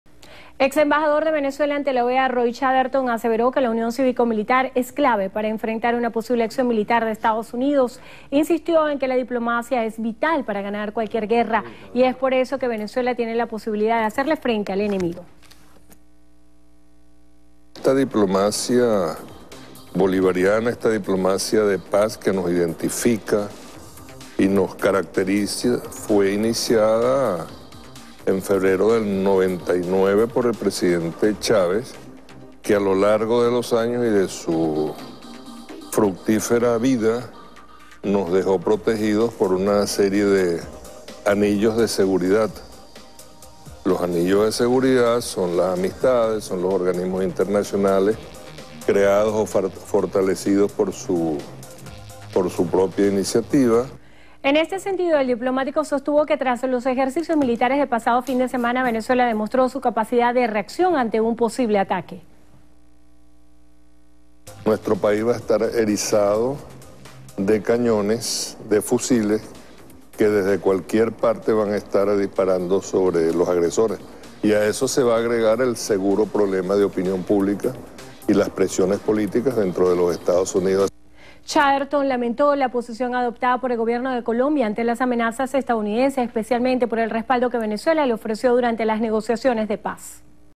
Chaderton: Dirigentes de la MUD le han declarado la guerra al país al apoyar intervención extranjera
El exembajador de Venezuela ante la OEA, Roy Chaderton condenó este martes las diligencias adelantadas por dirigentes de la Mesa de la Unidad Democrática (MUD) ante Estados Unidos, para favorecer una intervención extranjera contra Venezuela.